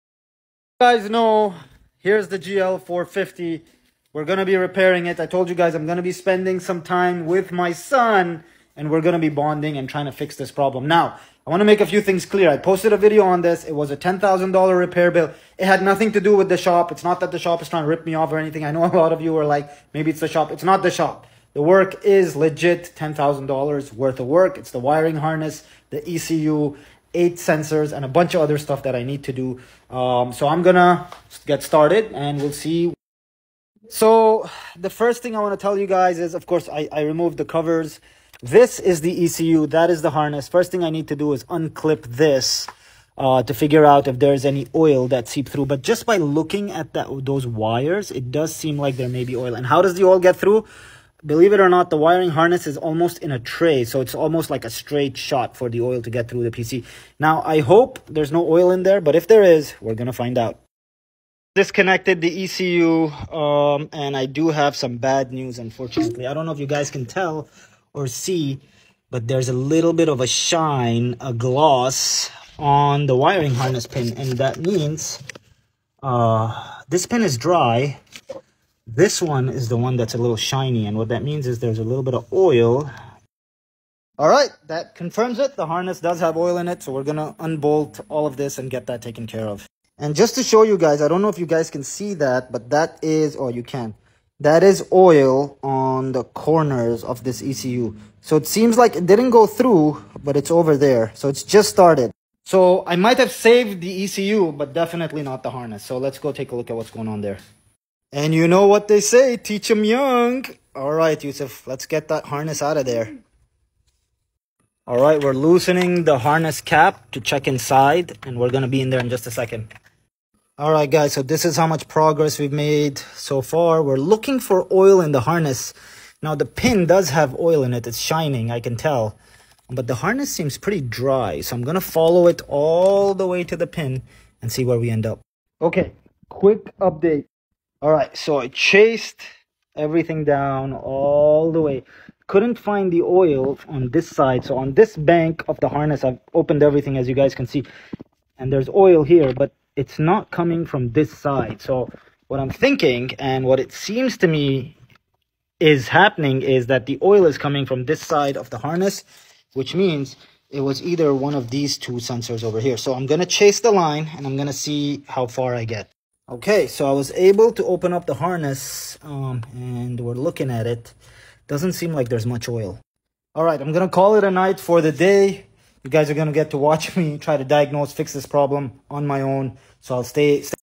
Can I Fix My Mercedes GL 450 Myself?
Will I be able to fix my Mercedes GL 450, or will I have to take it to the shop?